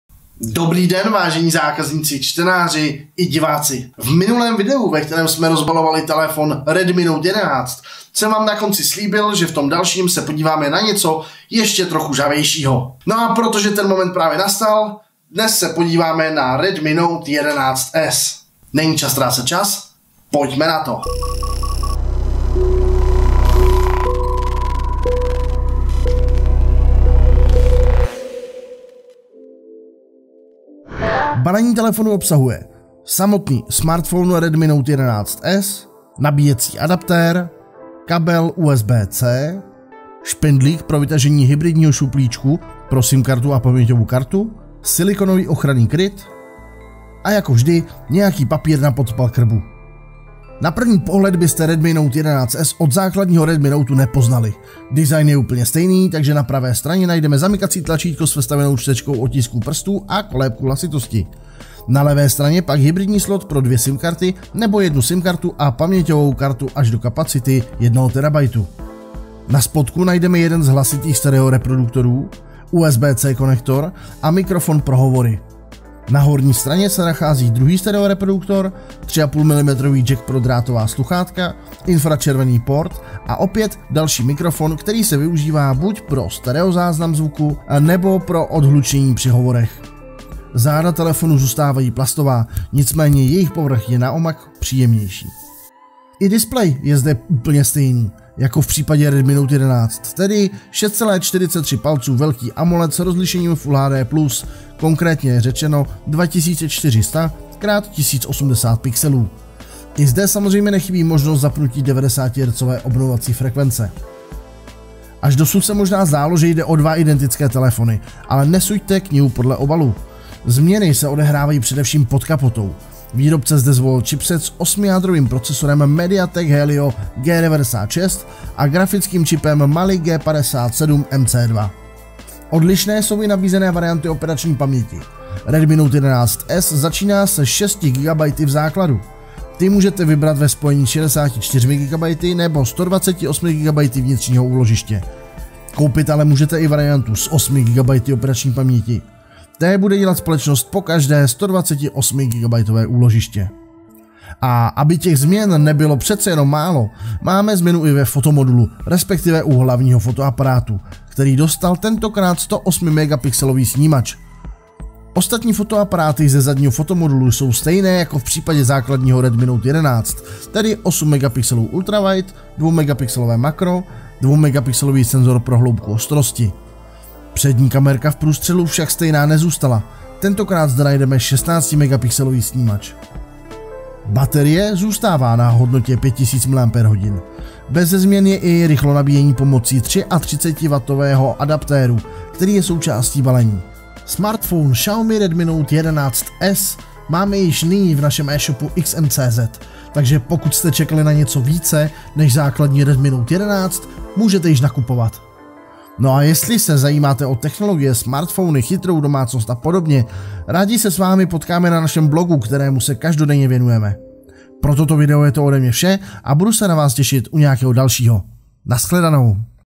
Xiaomi Redmi Note 11S - Unboxing a rychlý pohled
🛒 Xiaomi Redmi Note 11S lze objednat na našem českém i slovenském eshopu.
🔽 KOUPIT 🔽

Novinky, testy, recenze, poradna a srovnání ze světa Xiaomi

 @XM blog